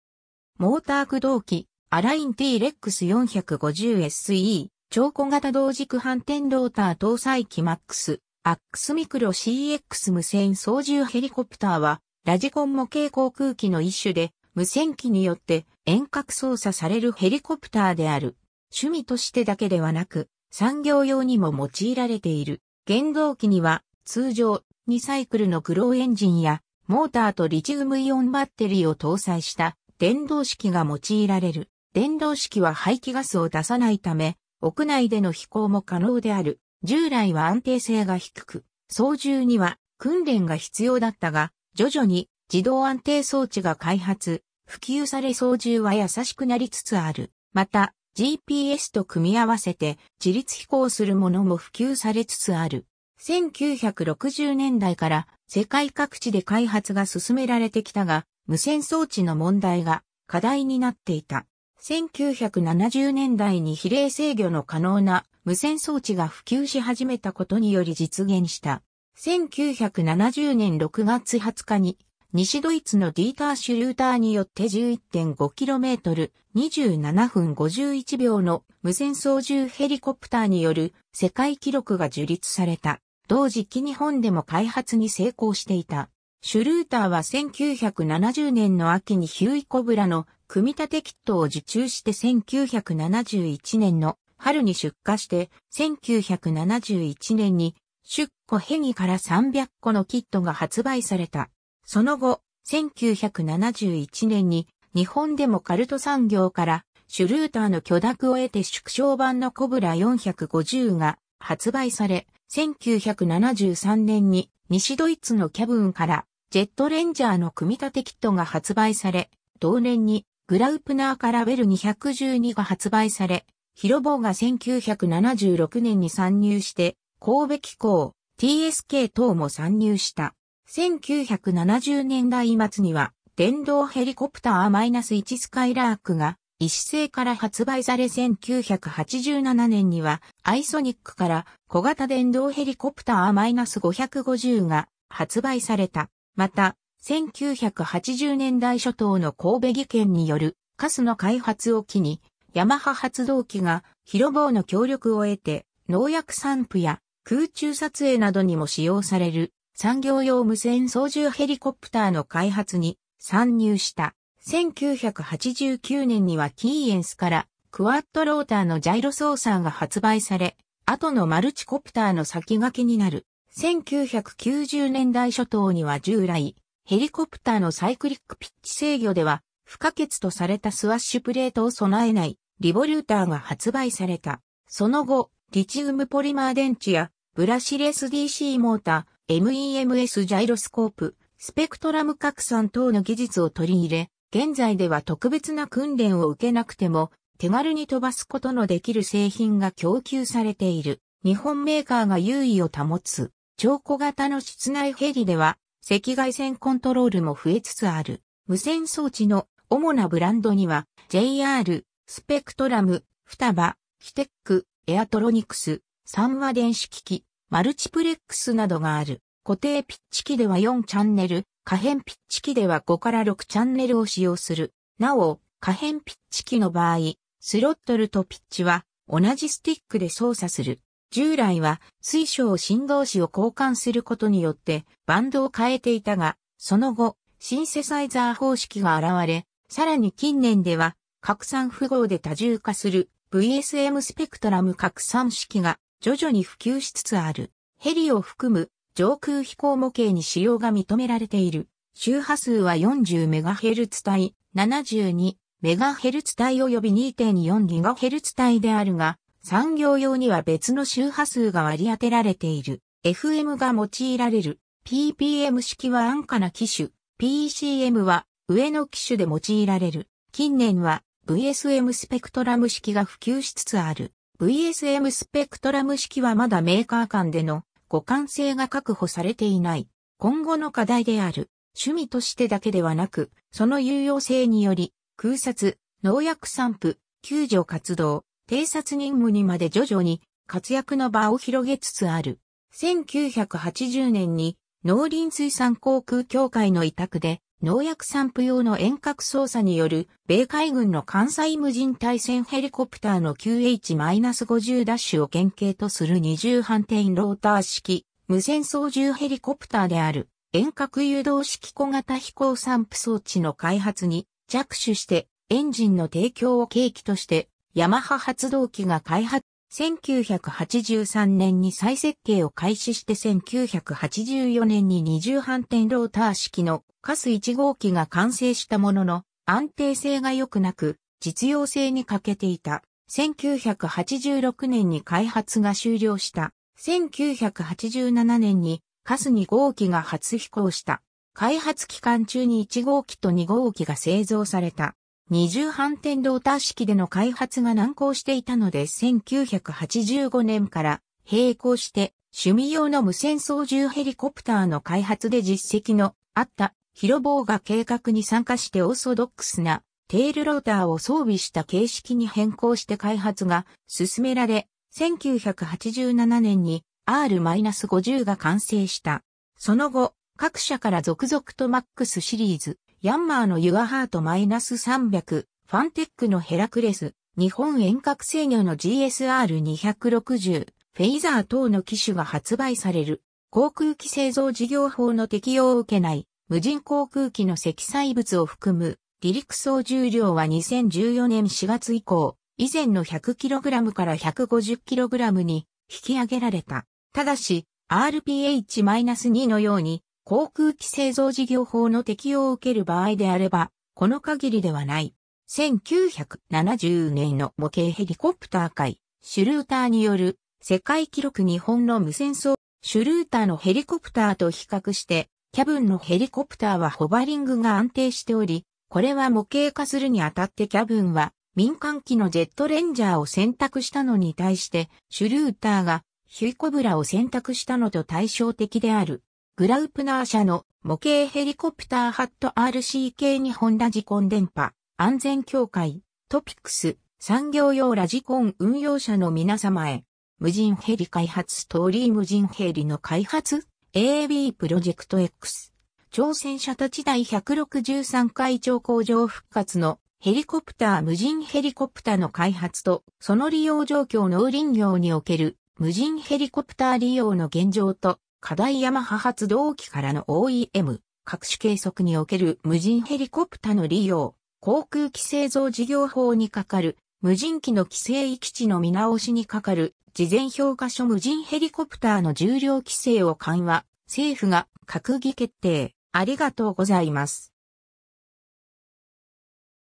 無線操縦ヘリコプター
モーター駆動機 Align T-rex 450SE 超小型同軸反転ローター搭載機 Max Axe Micro CX 無線操縦ヘリコプター（むせんそうじゅうヘリコプター、RC ヘリコプター）は、ラジコン模型航空機の一種で、無線機によって遠隔操作されるヘリコプターである。
趣味としてだけではなく、産業用にも用いられている。
原動機には通常、2サイクルのグローエンジンや、モーターとリチウムイオンバッテリーを搭載した電動式が用いられる。
電動式は排気ガスを出さない為、屋内での飛行も可能である。
 従来は安定性が低く、操縦には訓練が必要だったが、徐々に自動安定装置が開発、普及され操縦は易しくなりつつある。
また、GPSと組み合わせて自律飛行するものも普及されつつある。
 1960年代から世界各地で開発が進められてきたが、無線装置の問題が課題になっていた。
1970年代に比例制御の可能な無線装置が普及し始めたことにより実現した。
1970年6月20日に西ドイツのディーター・シュルーター(Dieter Schlüter)によって11.5km、27分51秒の無線操縦ヘリコプターによる世界記録が樹立された。
同時期日本でも開発に成功していた。
シュルーターは1970年の秋にヒューイ コブラの組み立てキットを受注して1971年の春に出荷して、1971年にSchuco-Hegiから300個のキットが発売された。
その後、1971年に日本でもカルト産業からシュルーターの許諾を得て縮小版のコブラ450が発売され、1973年に西ドイツのKAVANからジェットレンジャーの組み立てキットが発売され、同年にグラウプナーからベル212が発売され、ヒロボーが1976年に参入して、神戸機工、TSK等も参入した。
1970年代末には電動ヘリコプターEH-1スカイラークが石政から発売され1987年にはアイソニックから小型電動ヘリコプターEH-550が発売された。
また、1980年代初頭の神戸技研によるRCASSの開発を機にヤマハ発動機がヒロボーの協力を得て農薬散布や空中撮影などにも使用される産業用無線操縦ヘリコプターの開発に参入した。
1989年にはキーエンスからクワッドローターのジャイロソーサーが発売され、後のマルチコプターの先駆けになる。
1990年代初頭には従来、ヘリコプターのサイクリックピッチ制御では不可欠とされたスワッシュプレートを備えないリボリューターが発売された。
 その後、リチウムポリマー電池やブラシレスDCモータ、MEMSジャイロスコープ、スペクトラム拡散等の技術を取り入れ、現在では特別な訓練を受けなくても手軽に飛ばすことの出来る製品が供給されている。
 日本メーカーが優位を保つ。
超小型の室内ヘリでは赤外線コントロールも増えつつある。
無線装置の主なブランドには、JR, Spektrum, Futaba, Hitec, エアトロニクス, 三和電子機器, Multiplexなどがある。
 固定ピッチ機では4チャンネル（スロットル、エルロン、エレベーター、ラダー）、可変ピッチ機では5〜6チャンネル（スロットル、エルロン、エレベーター、ラダー、ピッチ、ジャイロ感度調整）を使用する。
なお、可変ピッチ機の場合、スロットルとピッチは同じスティック（日本で使用されているモード1送信機では右スティックの上下）で操作する。
従来は水晶振動子を交換する事によってバンド(帯域)を変えていたが、その後、シンセサイザー方式が現れ、さらに近年では拡散符号で多重化するVSMスペクトラム拡散式が徐々に普及しつつある。
ヘリを含む上空飛行模型に使用が認められている周波数は40MHz帯、72MHz帯および2.4GHz帯であるが、産業用には別の周波数が割り当てられている。
 FM(周波数変調)が用いられる。
PPM式は安価な機種、PCMは上の機種で用いられる。
 近年はVSMスペクトラム式が普及しつつある。
VSMスペクトラム式はまだメーカー間での互換性が確保されていない。
今後の課題である。
 趣味としてだけではなく、その有用性により、空撮、農薬散布、救助活動、偵察任務にまで徐々に活躍の場を広げつつある。
 1980年に(社)農林水産航空協会の委託で農薬散布用の遠隔操作による米海軍の艦載無人対潜ヘリコプターのQH-50 DASHを原型とする二重反転ローター式無線操縦ヘリコプターである遠隔誘導式小型飛行散布装置(RCASS)の開発に着手して、エンジンの提供を契機としてヤマハ発動...